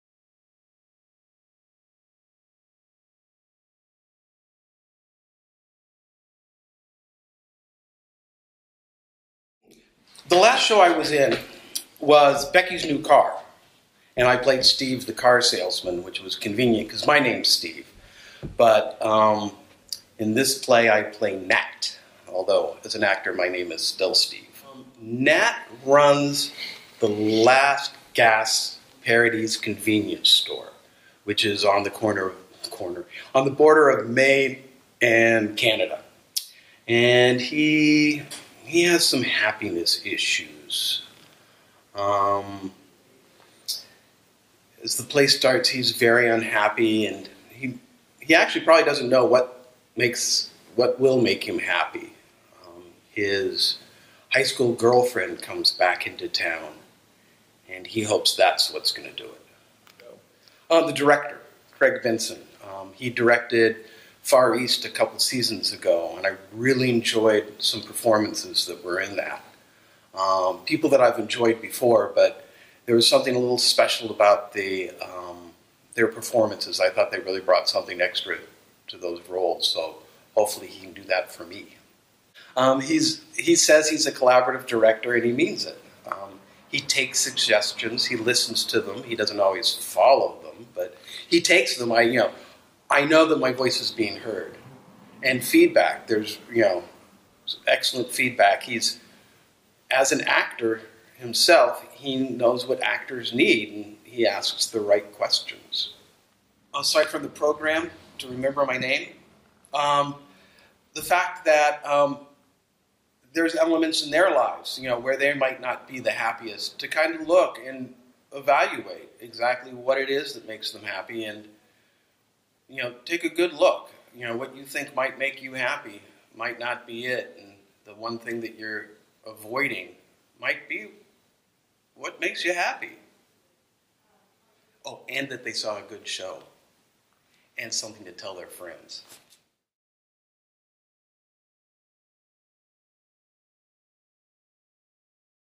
4 Questions for "Last Gas" Actor Steve Carter, Redwood Curtain Theatre, Eureka, CA
"Last Gas" plays Redwood Curtain August 27 to September 19, 2015
Thursdays, Fridays, Saturdays at 8pm. Sunday matinee on September 13 at 2pm.

About "Last Gas" by John Cariani:
Nat Paradis is a Red Sox-loving part-time dad who manages Paradis’ Last Convenient Store, the last convenient place to get gas—or anything—before the Canadian border to the north and the North Maine Woods to the west. When an old flame returns to town, Nat gets a chance to rekindle a romance he gave up on years ago. But sparks fly as he’s forced to choose between new love and old. Last Gas takes a hilarious and heartbreakingly hard look at love lost and found, and at what it means to “get back to happy.”